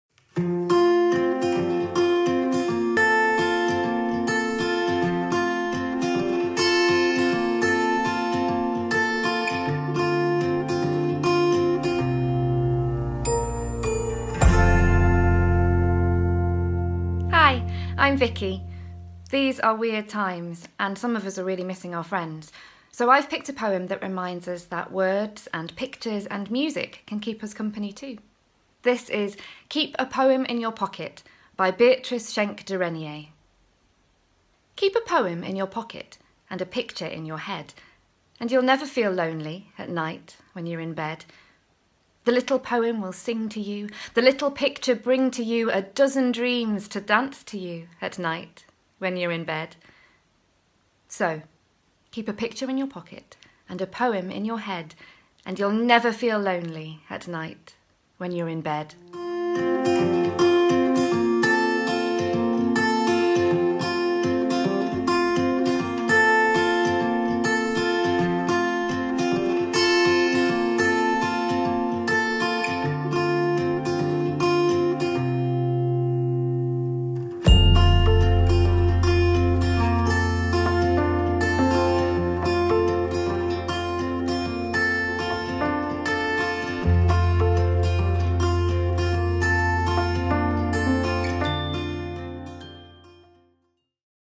FamilyPoems @ Home - Keep A Poem In Your Pocket by Beatrice Schenk de Regniers
At Watford Palace Theatre, we've been thinking about how poems can keep us company, and we wanted to share some that make us happy, with you.
In this video, actress Victoria Brazier reads 'Keep A Poem In Your Pocket' by Beatrice Schenk de Regniers.